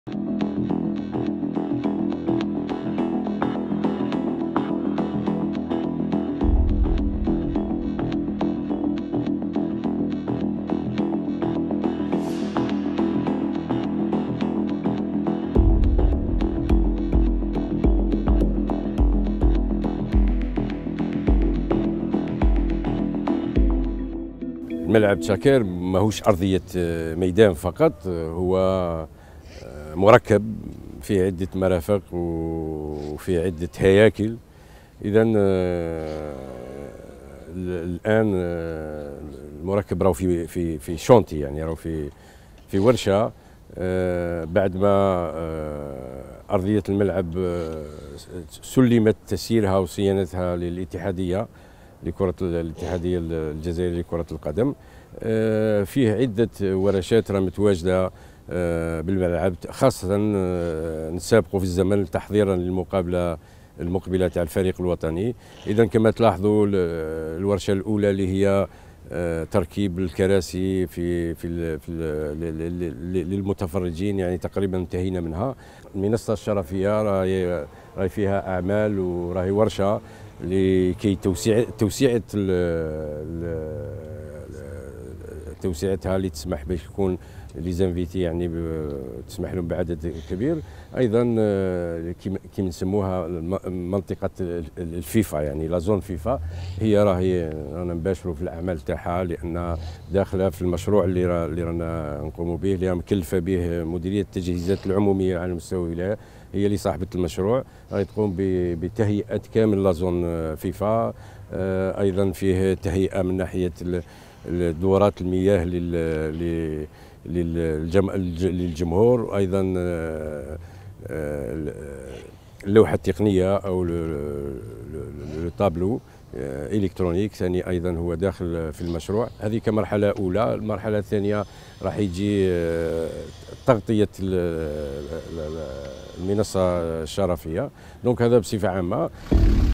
شاهدوا آخر أشغال ملعب تشاكر في حلته الجديدة قبل لقاء الكاميرون.. "الأرضية، المنصة الشرفية.. "🏟️
HD 10992/5000
SD 10921/27500/V